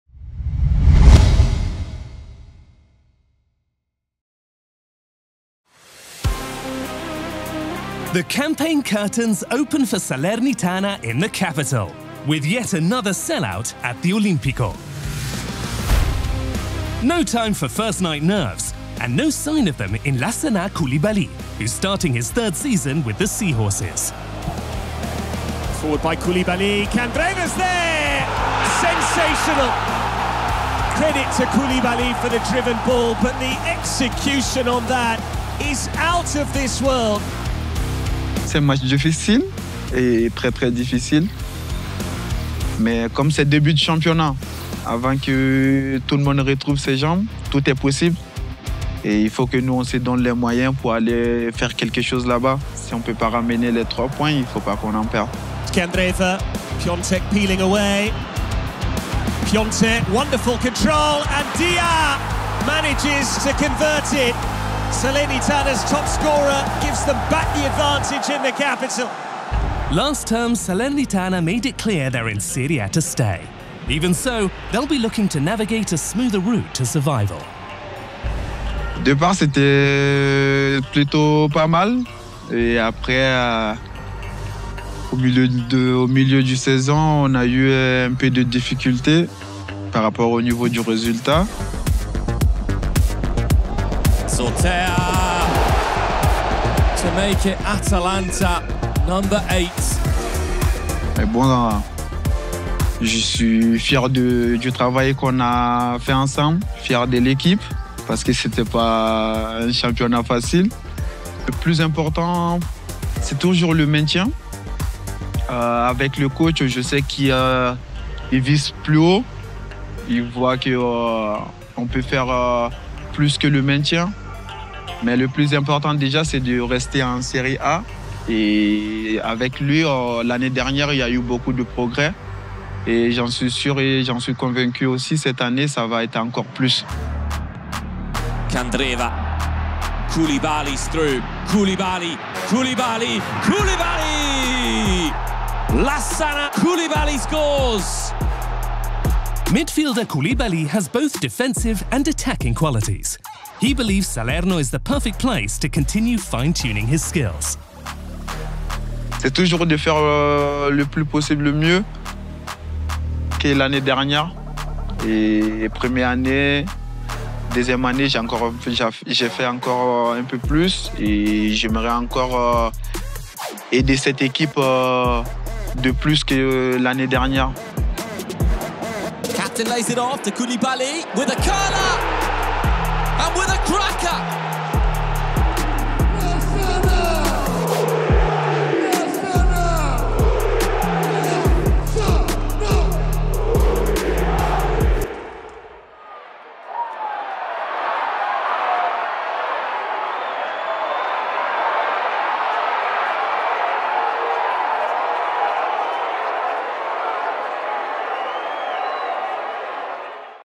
Salerno, Lassana's new home | A Chat with Coulibaly | Serie A 2023/24
Lassana Coulibaly, Salernitana's Malian midfielder, talks about his third season with the Granata, starting with his goal of saving himself and his ambition to further improve his qualities.